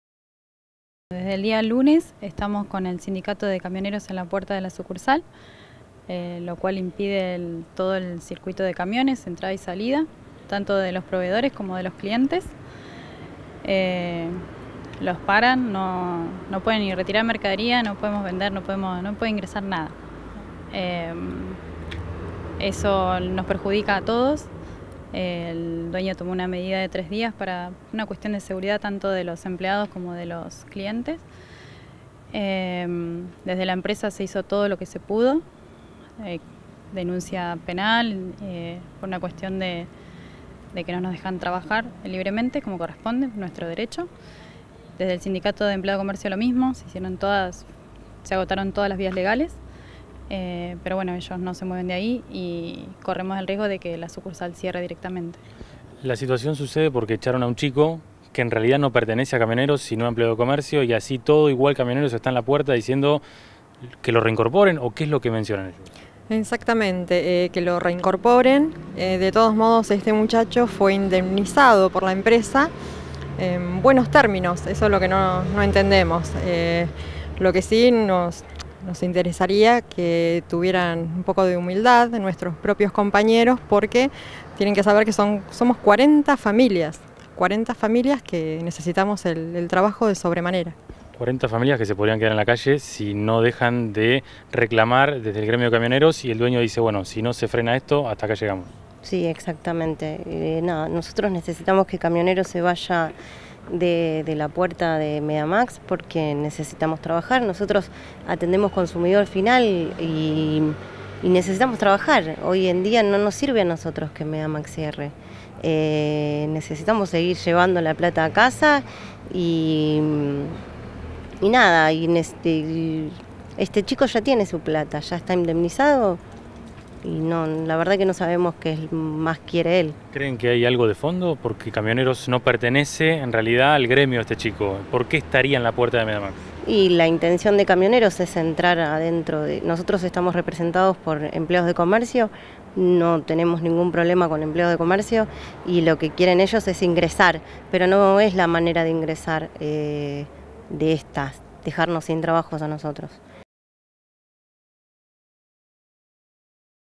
Por el reclamo de un gremio, todos los empleados podrían quedarse sin trabajo
Un grupo de trabajadores de la empresa Medamax reclamaron preocupados ante las cámaras de Canal Siete que su fuente laboral corre peligro según dichos del dueño de la empresa en Bahía Blanca.

Desde 2015 que viene la puja del Gremio de Camioneros para que algunos de los empleados puedan pertenecer al gremio. Hoy todos pertenecen a Empleados de Comercio. En las últimas horas un trabajador fue echado con indemnización. Y ahora Camioneros reclama que lo reincorporen y que otras 6 personas que tienen intenciones de cambiar de representación sindical lo puedan hacer.

Desde el miércoles la empresa, que está ubicada en Ruta 3 y Pacífico, no está teniendo actividad, por presencia de gremialistas y quien maneja la empresa, sostuvo que como todos viven de la venta, si no hay ingreso ni egreso de mercadería, corre riesgo que los 40 empleados se pueden quedar sin trabajo.